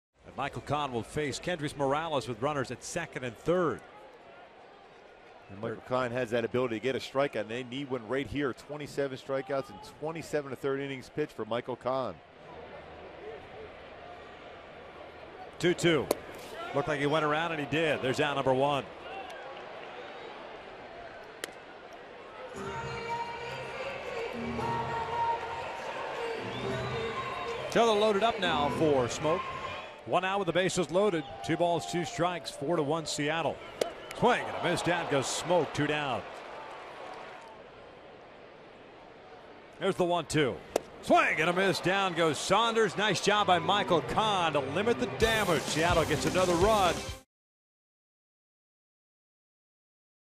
LAA@SEA: Kohns strikes out three to work out of fifth
7/14/13: Michael Kohn enters the game in relief and records three huge strikeouts to get the Angels out of the fifth inning